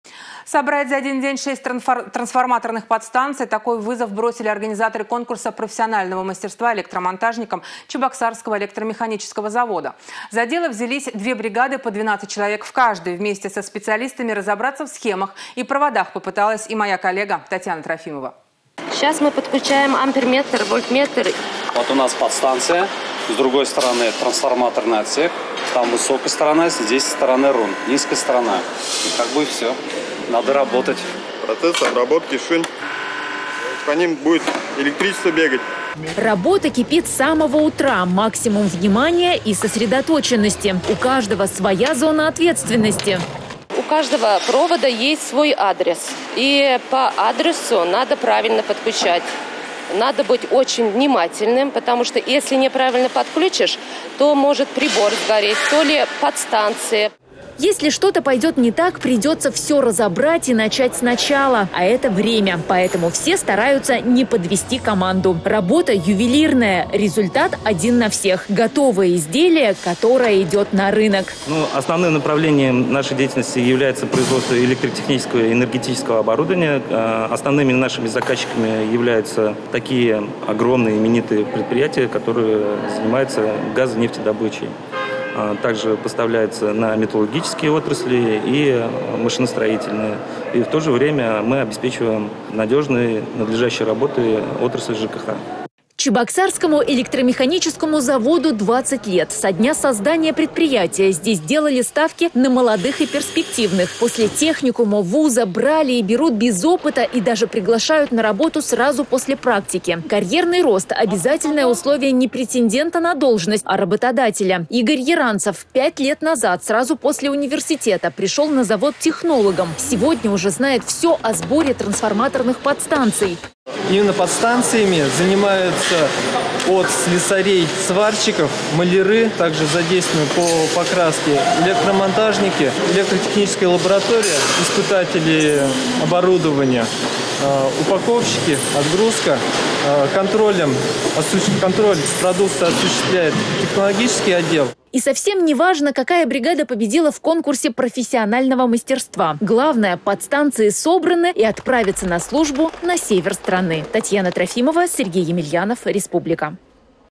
На Чебоксарском электромеханическом заводе прошел конкурс профессионального мастерства
Собрать за один день шесть трансформаторных подстанций. Такой вызов бросили организаторы конкурса профессионального мастерства электромонтажникам Чебоксарского электромеханического завода. За дело взялись две бригады по 12 человек в каждой. Вместе со специалистами разобраться в схемах и проводах попыталась и моя коллега Татьяна Трофимова.